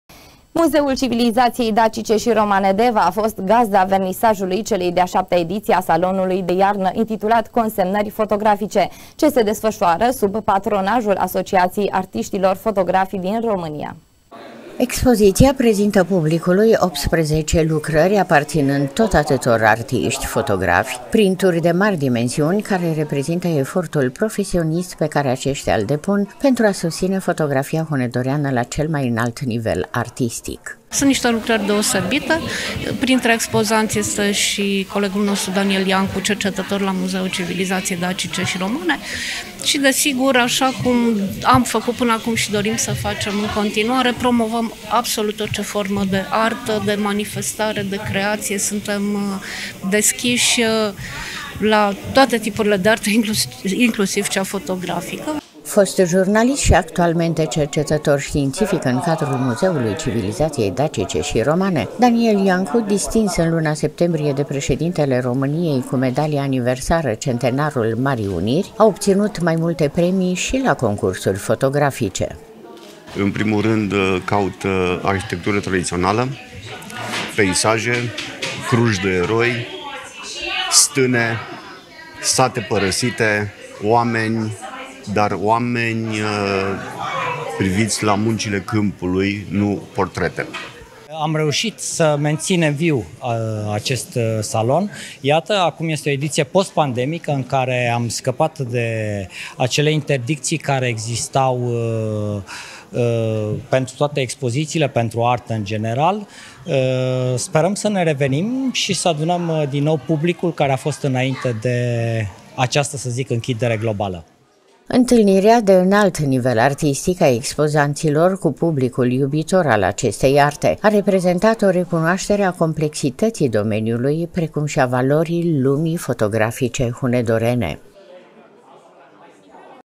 ”CONSEMNĂRI FOTOGRAFICE”, LA MUZEUL CIVILIZAȚIEI DACICE ȘI ROMANE DEVA
Muzeul Civilizației Dacice și Romane Deva a fost gazda vernisajului celei de-a șasea ediții a Salonului de iarnă intitulat „Consemnări Fotografice” ce se desfășoară sub patronajul Asociației Artiștilor Fotografi din România.